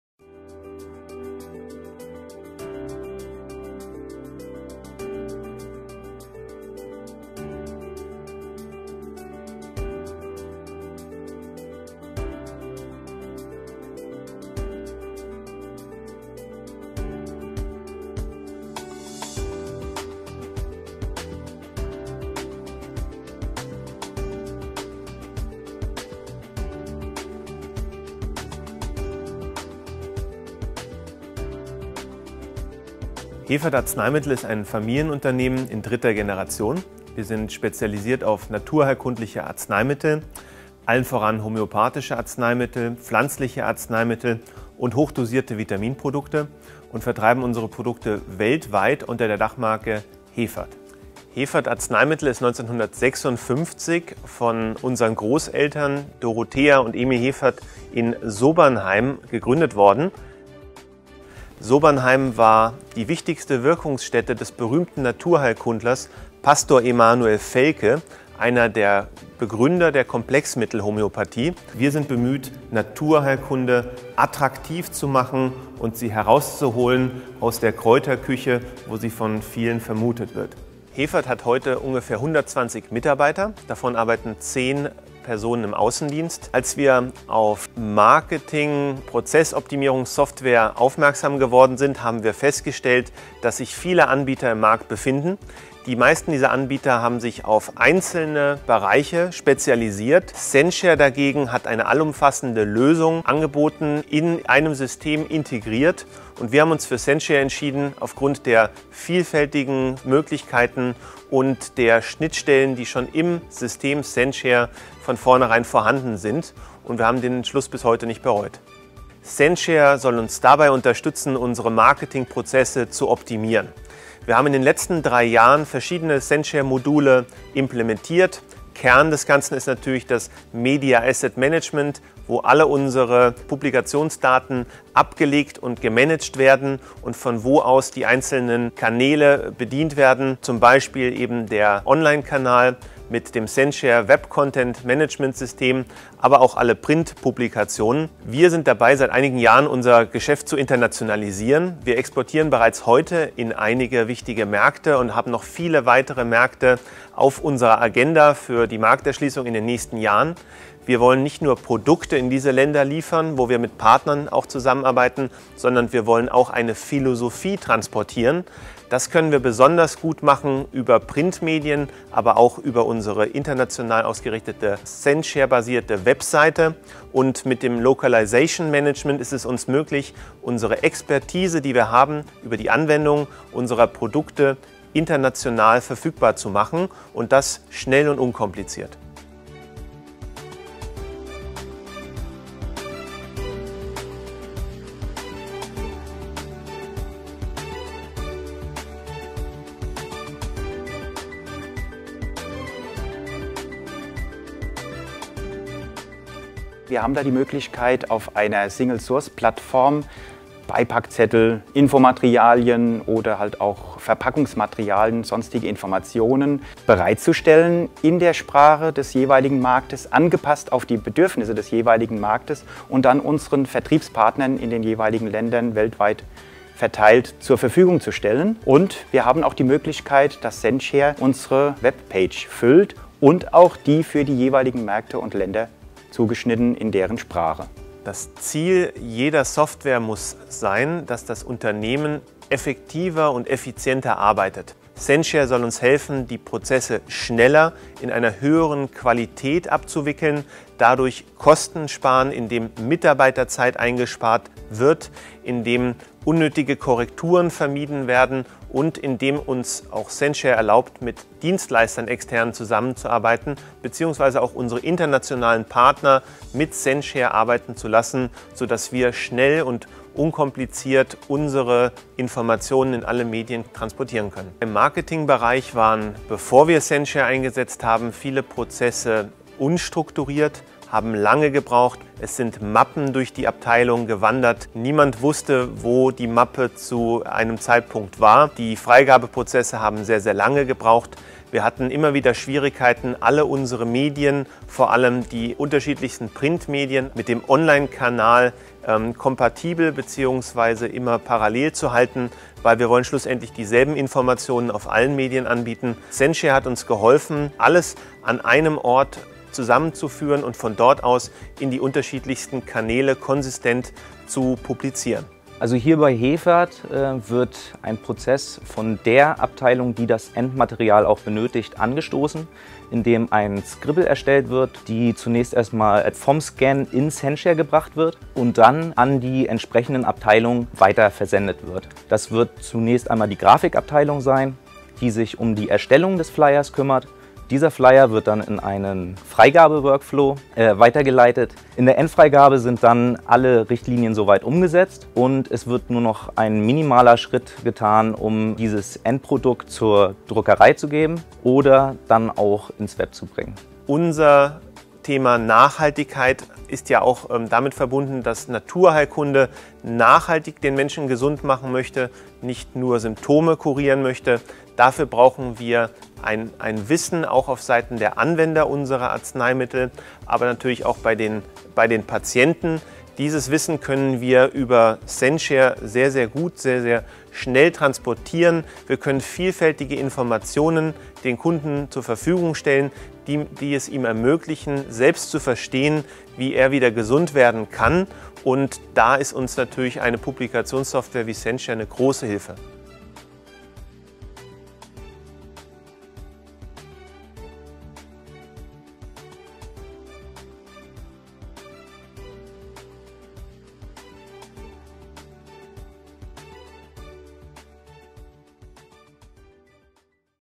Censhare Case Study - Hevert Arzneimittel GmbH & Co. KG
Die richtige Dosis Software!
Hevert-Arzneimittel gehört weltweit zu den zehn bedeutendsten Homöopathie-Herstellern. Das Unternehmen verfügt mit über 100 Arzneimitteln über ein breites Produktportfolio und deckt damit nahezu alle naturheilkundlich relevanten Therapiebereiche ab. Seit der Gründung im Jahr 1956 ist Hevert-Arzneimittel ein unabhängiges Familienunternehmen und wird heute in dritter Generation von Mathias Hevert geführt. Mit 120 Mitarbeitern konnte das Unternehmen seinen Umsatz 2011 um 6 Prozent auf 16,5 Mio. Euro steigern und liegt damit über der Marktentwicklung im rezeptfreien Markt. Die kontinuierliche Internationalisierung bildet einen wichtigen Baustein des nachhaltigen Unternehmenserfolgs.